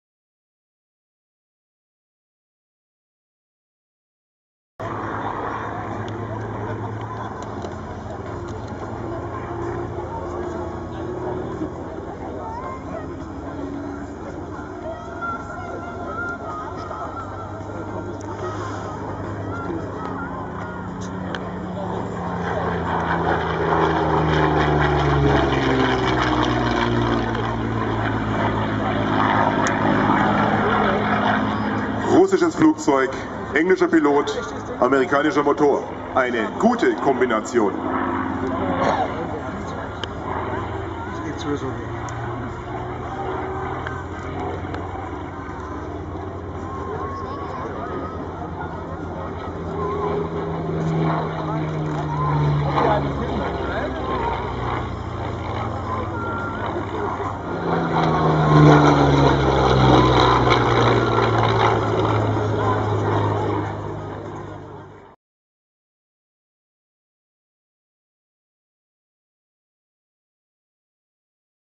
Yakovlev Yak 3 Flying at Hahnweide Vintage Air Show 2011
The Yakovlev Yak-3 manufactured by Yakovlev is a World War II Soviet fighter aircraft developed from the Yak-1. The Yak-3 was one of the smallest and lightest major combat fighters fielded by any combatant during the war, its high power-to-weight ratio gave it excellent performance. The Yak-3 dominated the skies over the battlefields of the Eastern Front during the closing years of World War II. The maiden flight of the Yak-3 took place on the 12th of April 1941 and it went into service in 1944. Primary users were the Soviet Air Force, Free French Normandie-Niemen, Air Force of the Polish Army and the SFR Yugoslav Air Force. There were approx. 4,848 Yak-3 produced. The Yak-3 retired from active service in 1956.